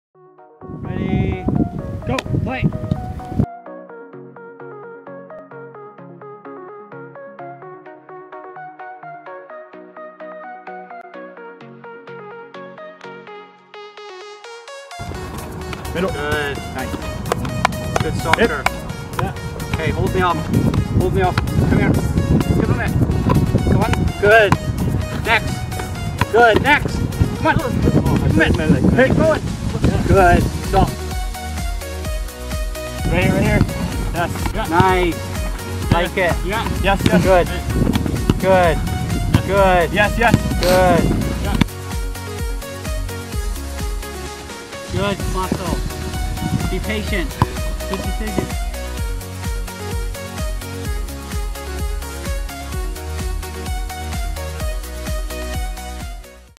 Elite Training
Terry Dunfield Soccer Academy offers elite 1 on 1 coaching that you might not be getting at club level. We will give you the skills and techniques that were the foundation of Terry's 17 year international career.